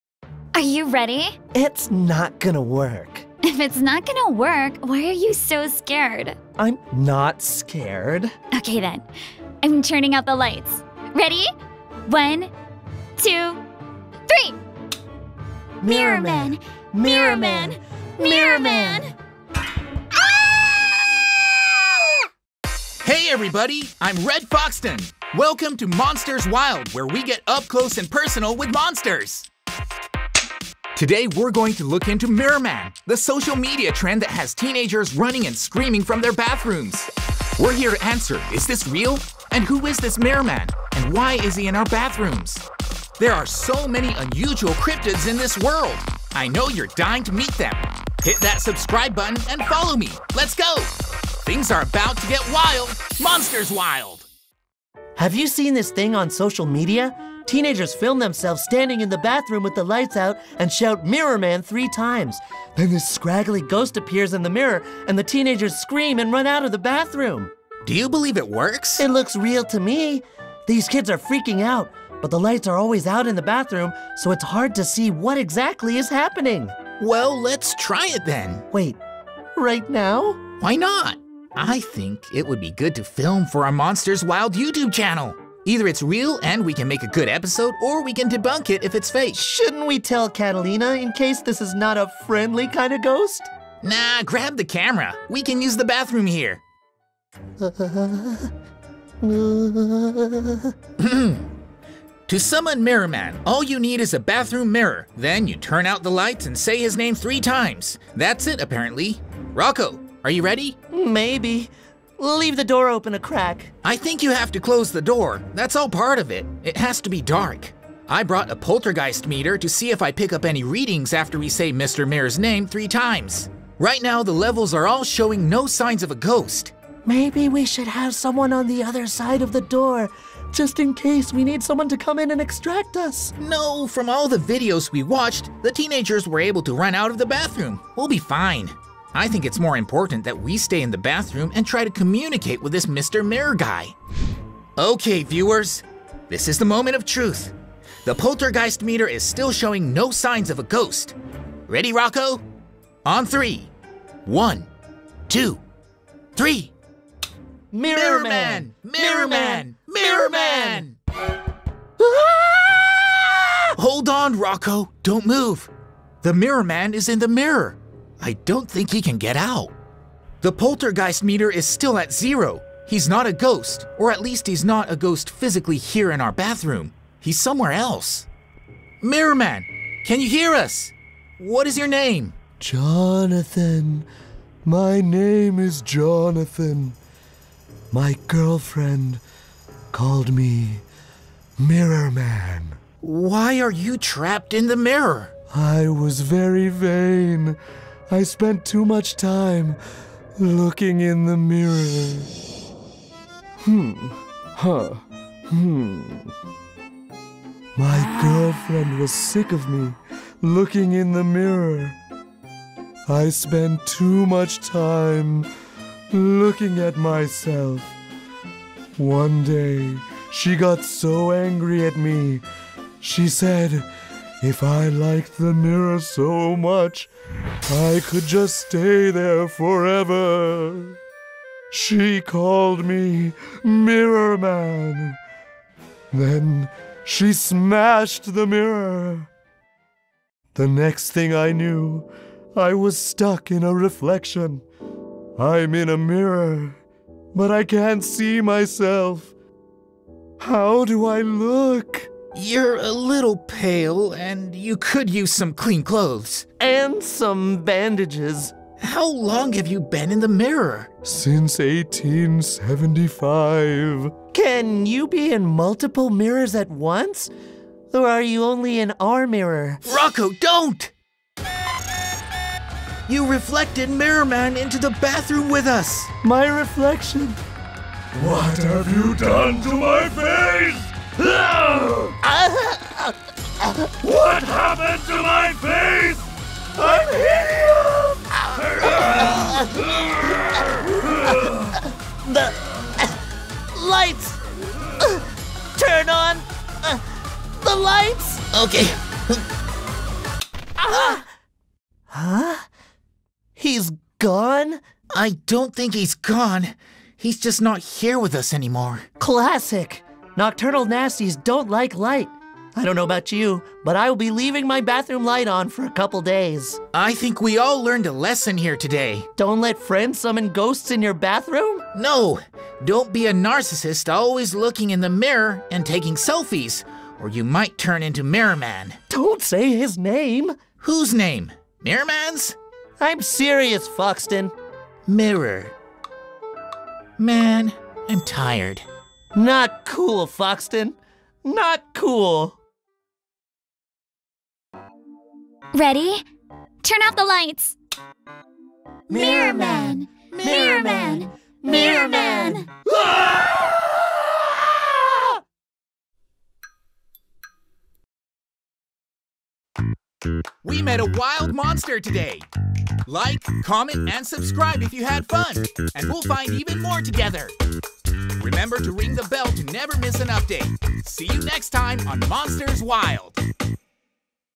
Mirror Man - a Scary Urban Legend｜Original Cartoon Animation
Before telling the terrifying urban legend, please answer the following:
Is it possible to love looking at yourself in the mirror too much?
What if you were stuck that way, cursed forever to be in a mirror?
The ghost of Mirror Man has been scaring teenagers for years...

Hey guys, is there a monster you want to know more about?

Production by Plott Inc.